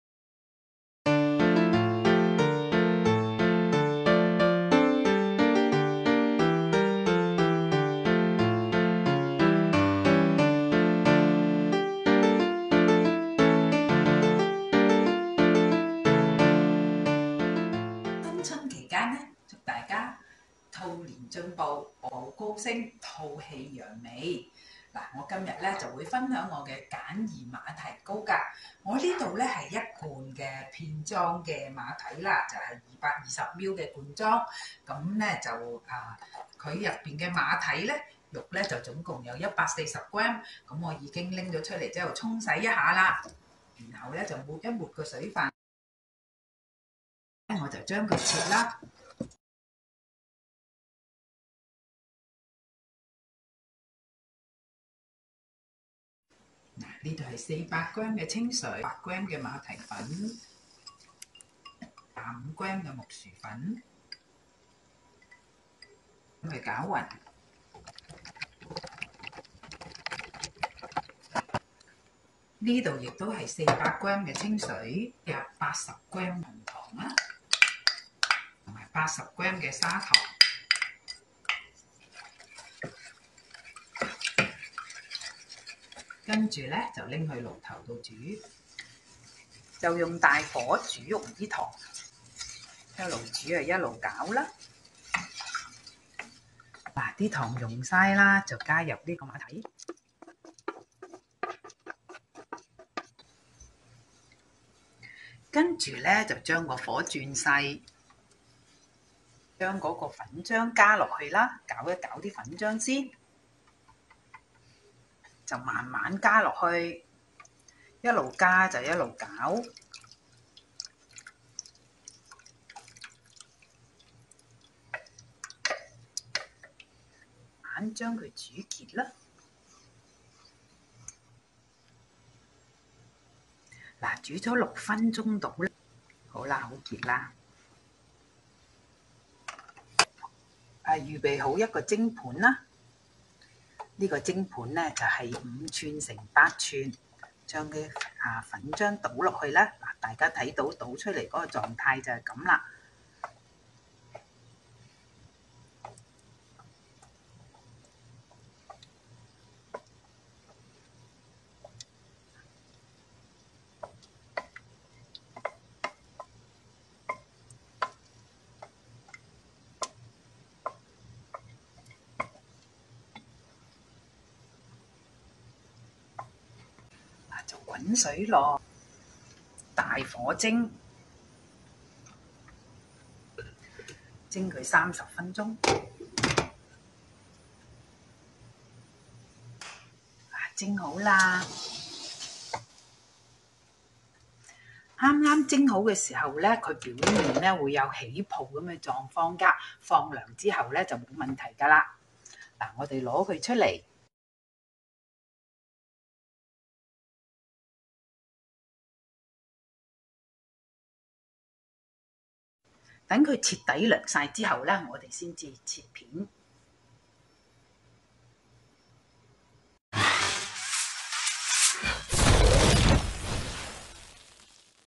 簡易黃金馬蹄糕 Simple Golden Water Chestnut Cake🧧晶瑩通透,清甜Q軟Easy, Crystal Clear,Sweet Soft with a Gummy Touch
材料(5"x8"糕) ingredients for a 5"x8" cake:

1. 140g 罐裝馬蹄片,沖洗後隔去水份抹乾, 然後切碎
canned sliced water chestnuts, rinse n drain the water n wipe dry, then choose it

2. 糖水 syrup
400g 水 water
80g 糖 sugar
80g 紅糖 brown sugar

3. 粉漿 the batter
拌勻以下材料
blend well the following ingredients
400g 水 water
100g 馬蹄粉 water chestnut starch
25g 木薯粉

做法 procedures :
1. 於鍋內,大火煑溶糖於400g 水中
melt the sugar in a pot of 400g water
2. 加入馬蹄,拌拌
add water chestnuts n stir it
3.轉細火, 拌拌粉漿,慢慢加入糖水中,邊加邊攪
turn to low heat, stir the batter n slowly add to the syrup, keep stirring while cooking
4. 煑成糊狀約6分鐘
cook into a paste in about 6 mins
5. 倒入已掃油5"x8"糕盤
pour into a lightly oiled 5"x8" cake pan
6. 滾水落,大火蒸30分鐘
steam on boiling water on high heat for 30 mins
7. 剛蒸好,糕面起泡泡,取出放涼後便平滑
the cake looks bubbling. Take it out n the cake top will smooth out after cooling down
8. 徹底放涼後才脫模切片
after completely cooled down, remove from pan n slice